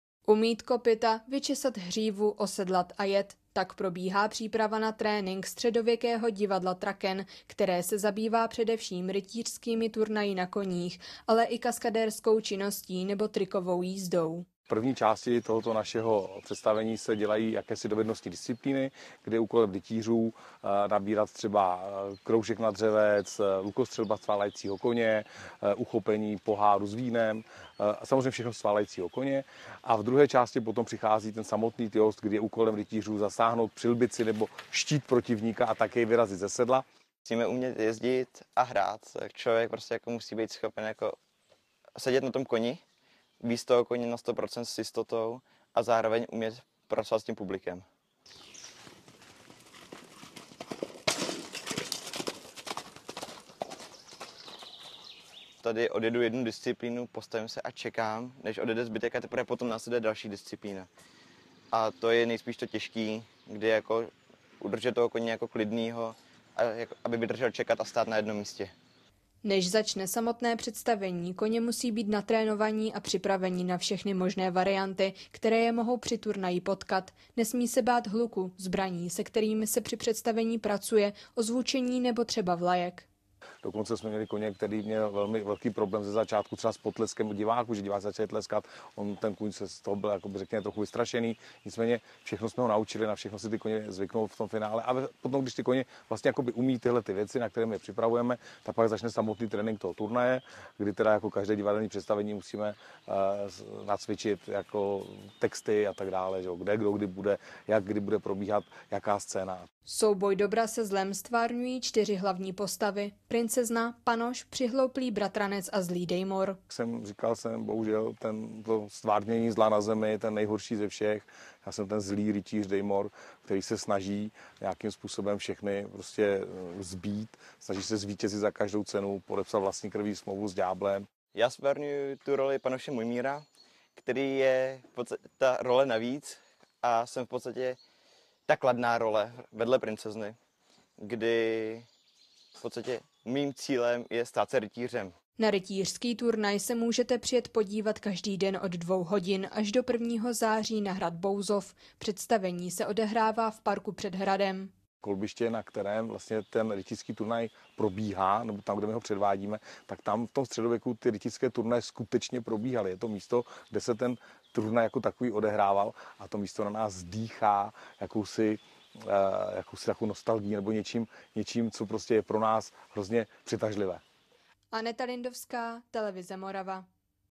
Hrad Bouzov ovládli rytíři na koních
Umýt kopyta, vyčesat hřívu, osedlat a jet. Tak probíhá příprava na trénink středověkého divadla Traken, které se zabývá nejen rytířskými turnaji na koních, ale i kaskadérskou činností nebo trikovou jízdou. Než začne samotné představení pro diváky, koně musí být natrénováni a připraveni na všechny možné varianty, které je mohou při turnaji potkat. Nesmí se bát hluku, zbraní, se kterými se při představení pracuje, ozvučení nebo třeba vlajek. Každé divadelní představení musí dokonale nacvičit včetně textů. Kde kdo bude pohybovat, kdy bude probíhat jaká scéna.

Souboj dobra ze zlem tvrárňují čtyři hlavní postavy – princezna, panoš, přihlouplý bratranec a zlý Dejmora. Na rytířský turnaj se můžete přijet podívat každý den od čtrnácti hodin až do 1. září na hrad Bouzov. Představení se odehrává v parku před hradem.